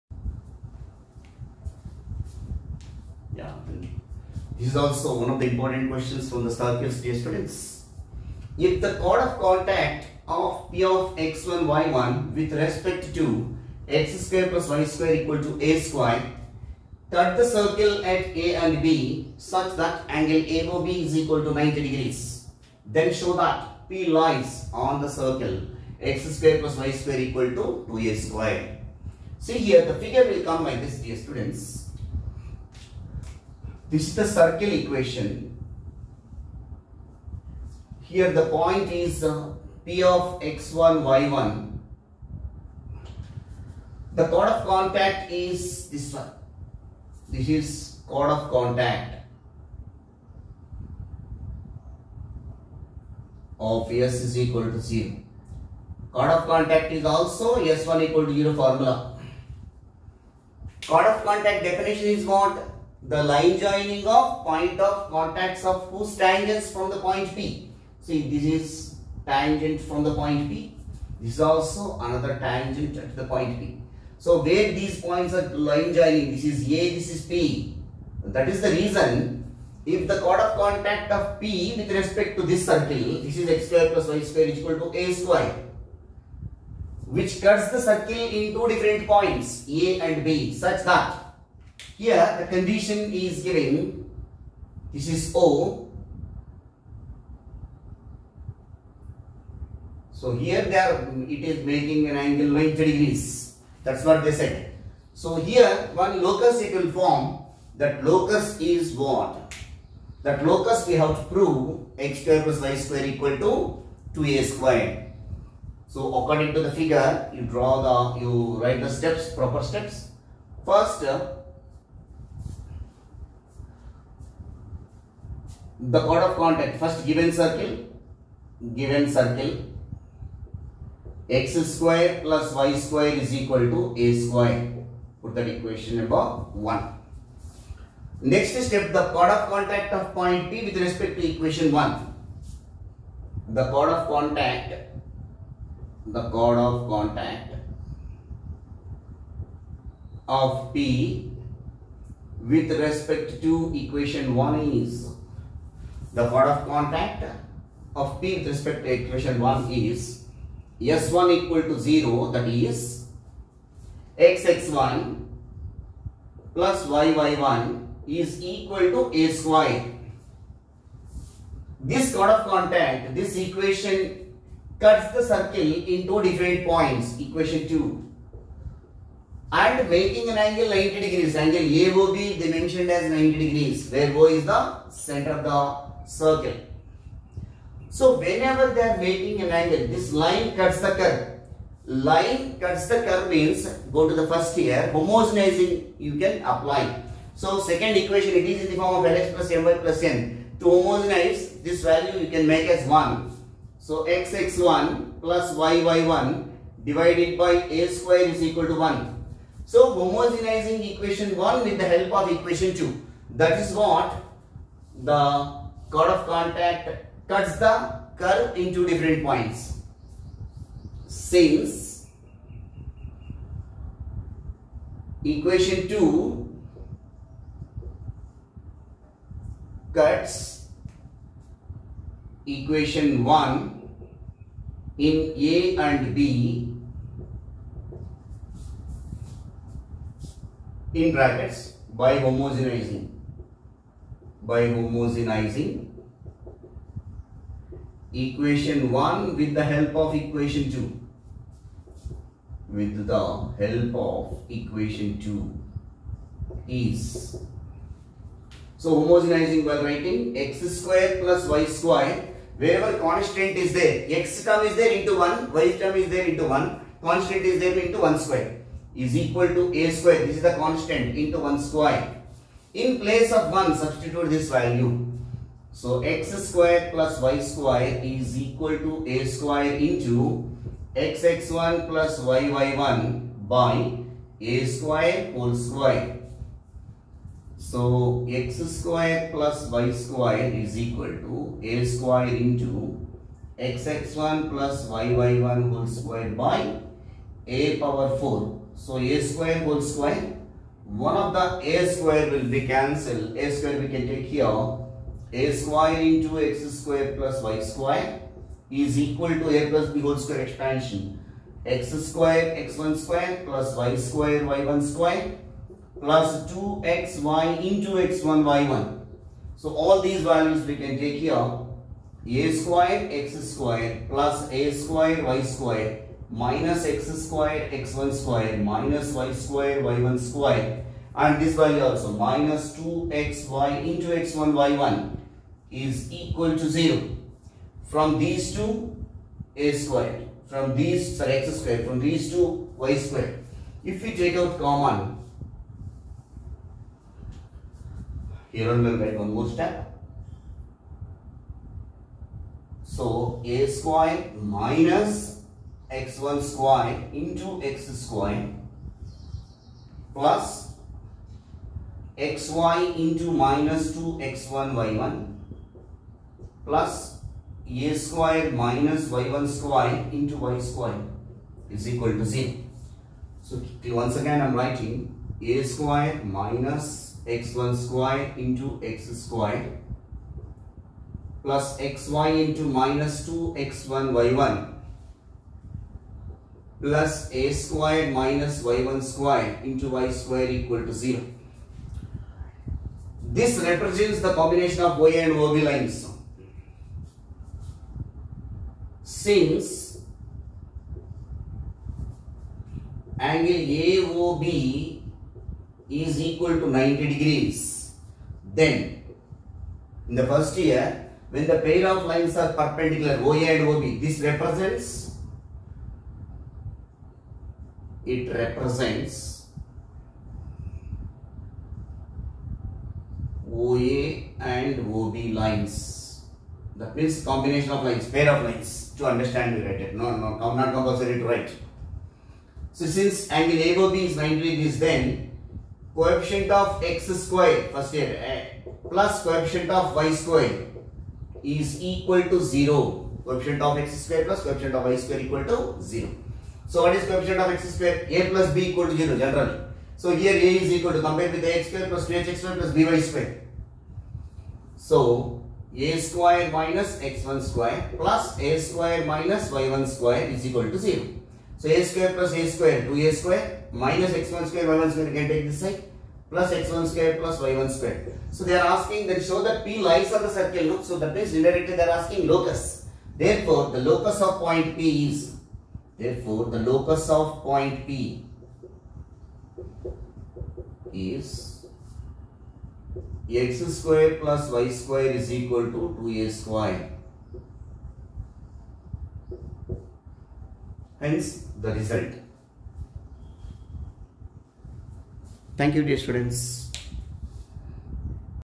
If the chord of contact of P w.r.t circle x²+y²=a² cut the circle at A and B such that angleAOB=90⁰
If the chord of contact of P w.r.t circle x²+y²=a² cut the circle at A and B such that angle AOB=90⁰ then show that P lies on the circle x²+y²=2a².